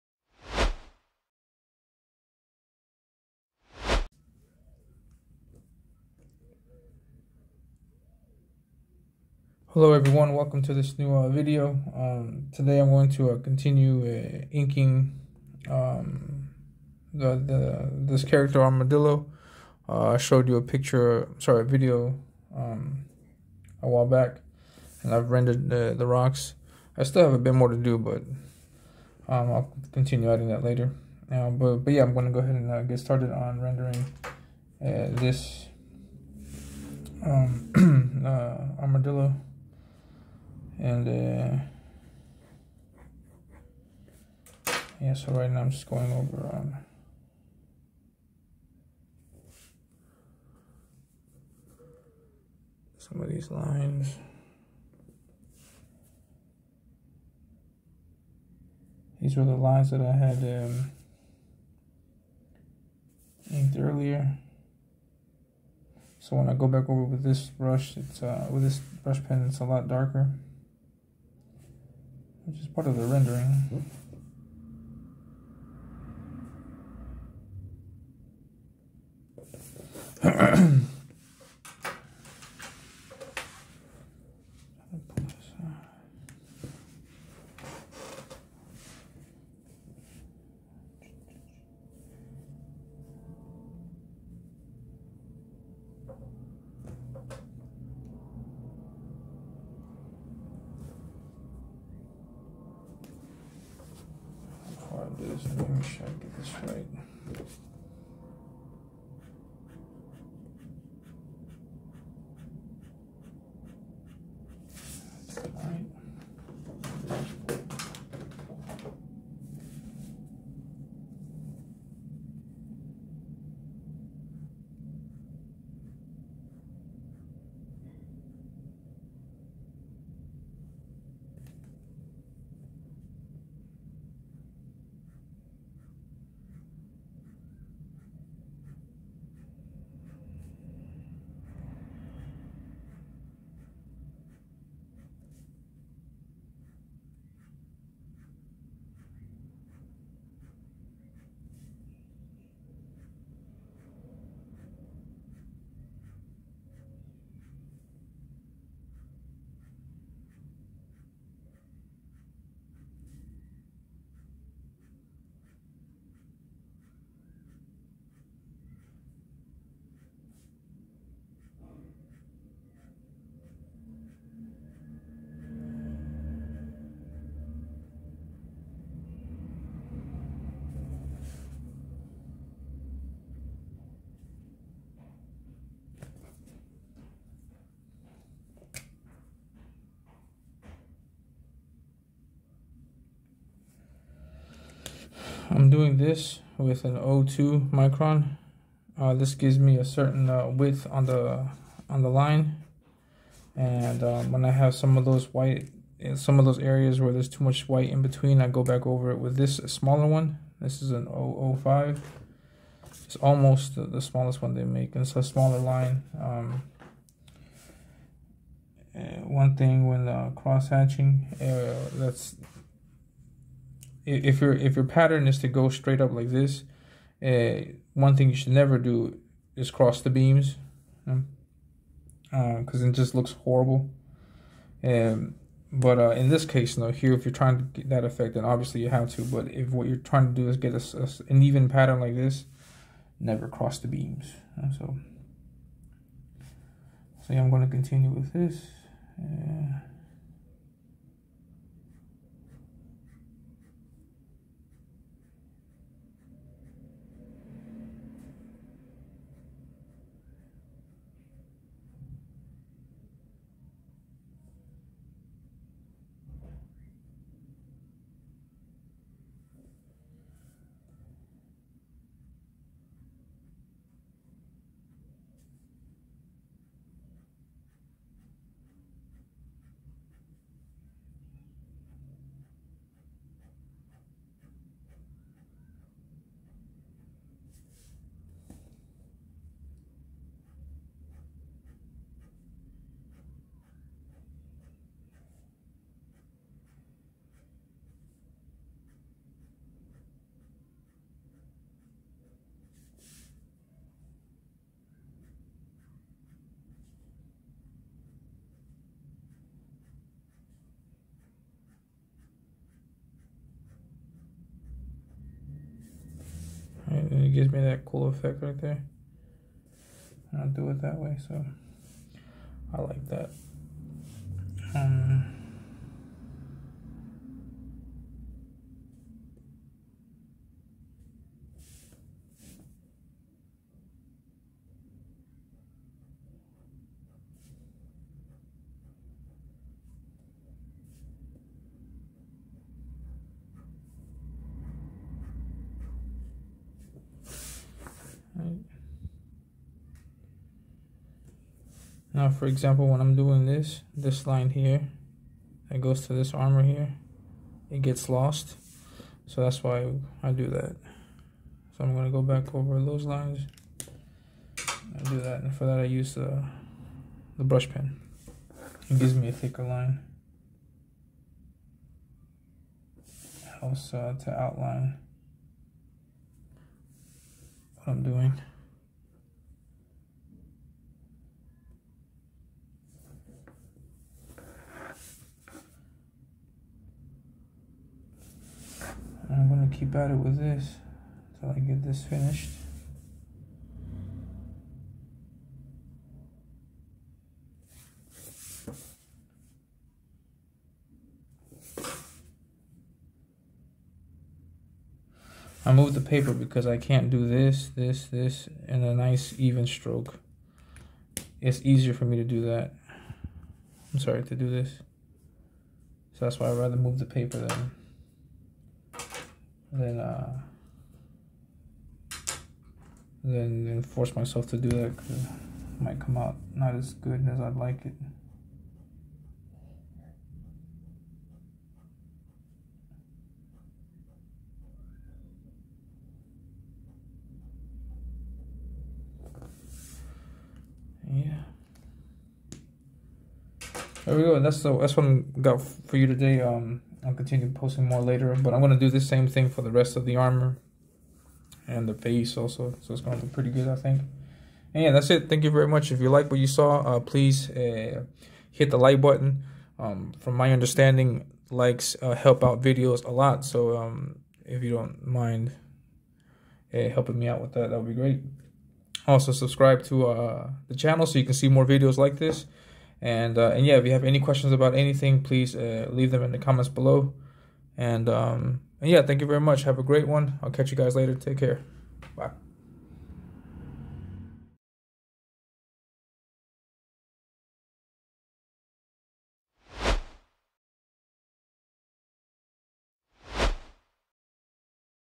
Rendering Armadillo
In this video, I'll show you my process for rendering Armadillo, a character  you'll be able to see in the upcoming Swarm comic from King Toad Comics.

And the King Toad Etsy Shop, where you can buy comics and original art from Joe Ball!!